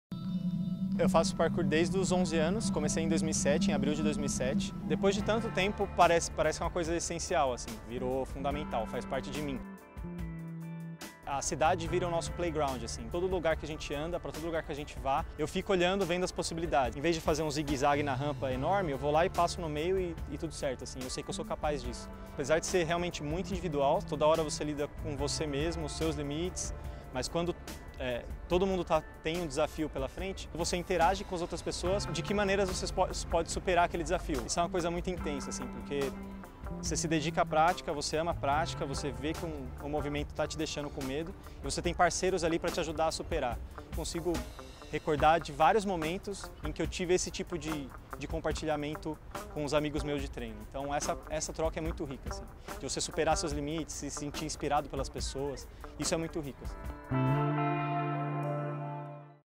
Web Série 3 : Parkour | Sesc Verão | Esporte para todos
Este vídeo é o terceiro de uma web série de seis que apresenta atividades do Sesc Verão 2019.

Os vídeos integram a reportagem realizada pela Revista E, edição de fevereiro de 2019 sobre a ocupação dos espaços públicos para práticas esportivas.

Produção: Bela Baderna Filmes